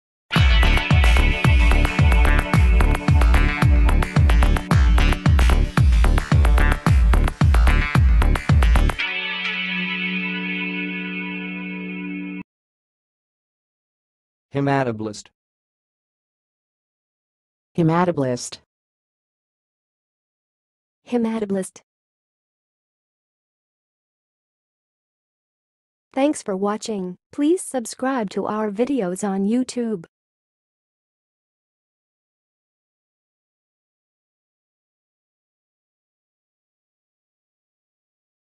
How to say "haematoblast"! (High Quality Voices)
Watch in this video how to say and pronounce "haematoblast"!